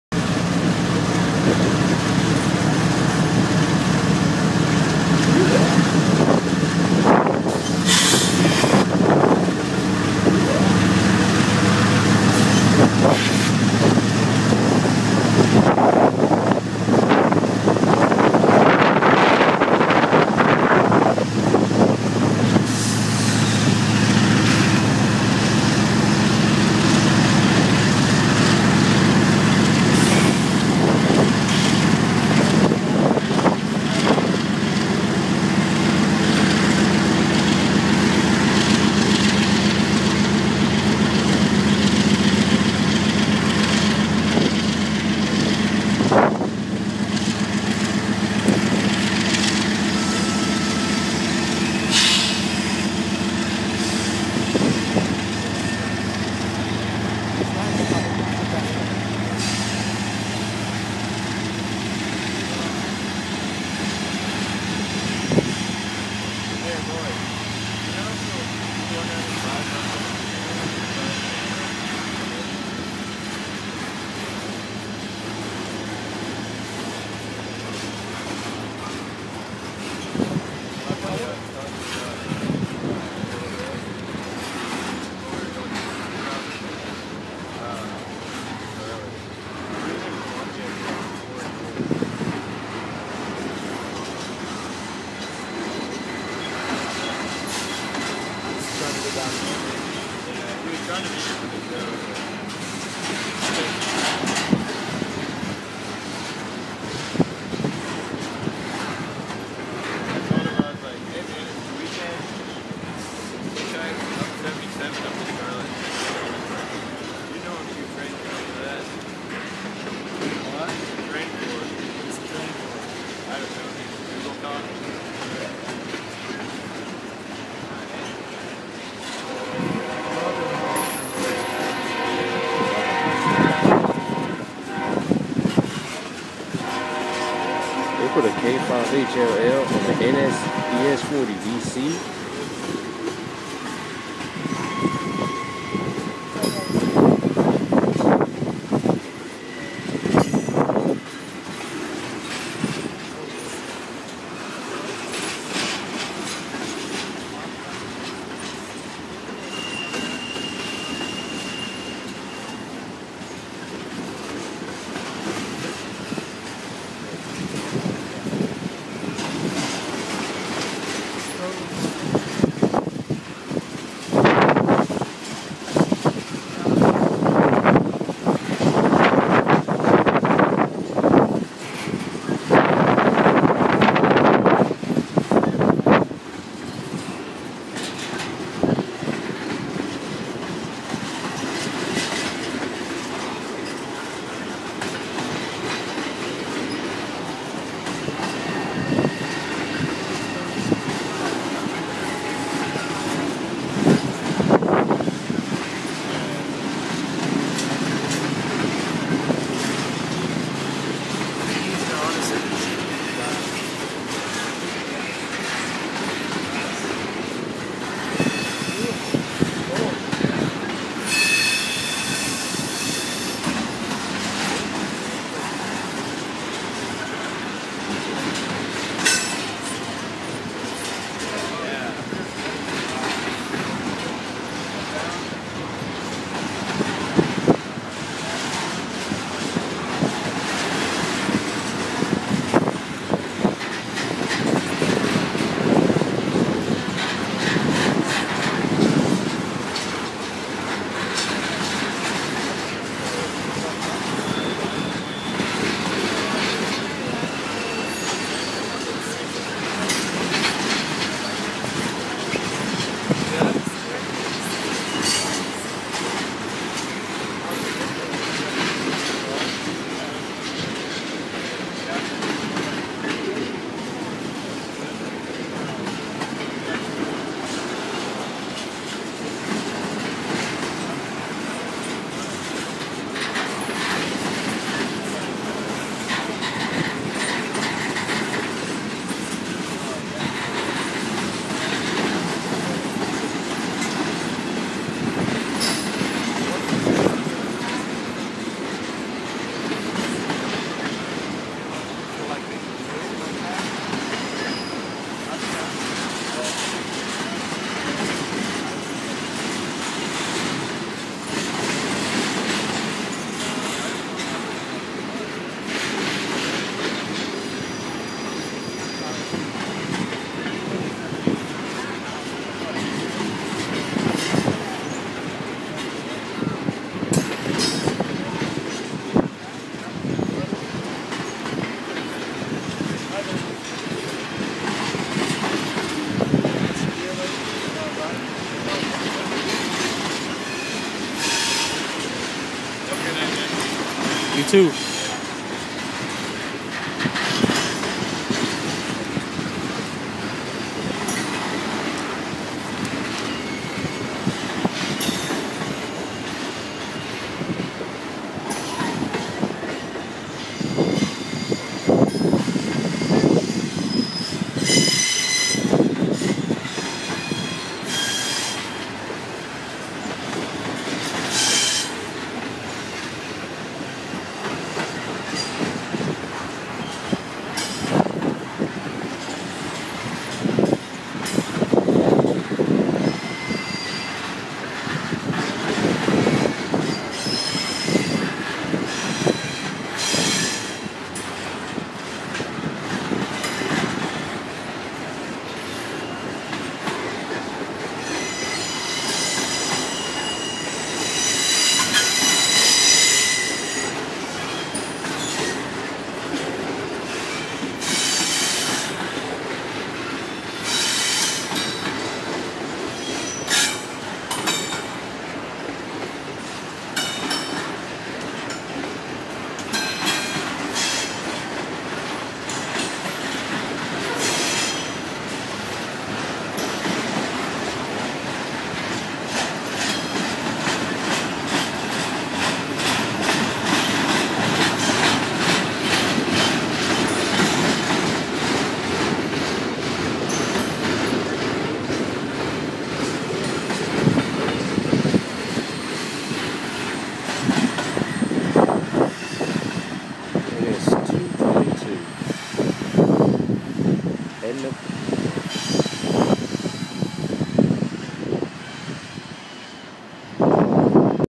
A Day at Howell Interlocking 10/27/12 Part 20: NS 222 Finale
The final treat is the departure of NS 222. Listen to those 12 Cylinder GEVOs wind as the train departs the junction, and the rare K5HLL on the NS ES40DC.

7:00PM
NS 222 (Atlanta, GA - Charlotte, NC)
Northbound Intermodal
Atlanta, GA
Howell Interlocking

NS ES40DC 7543 with K5HLL